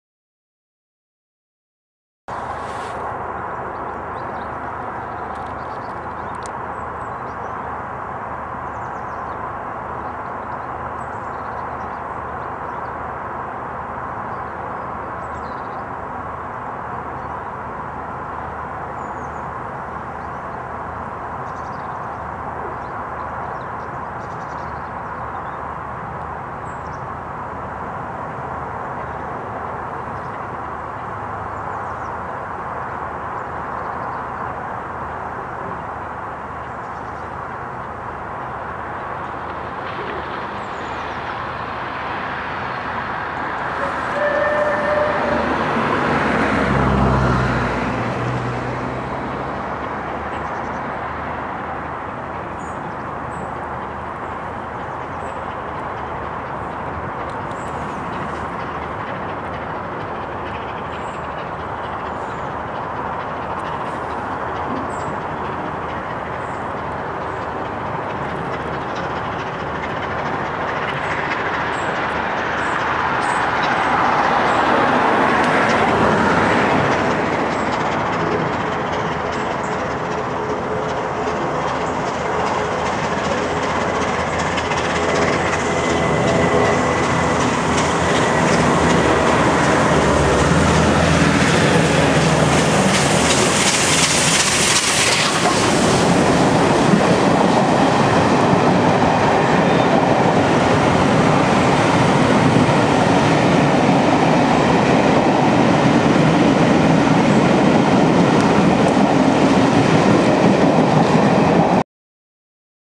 34067, Tangmere near Lyne on 6th October 2011
SR Battle of Britain Class Pacific, 34067, Tangmere accelerates away from Virginia Water with The Cathedrals Express from London to Bristol on 6th October 2011.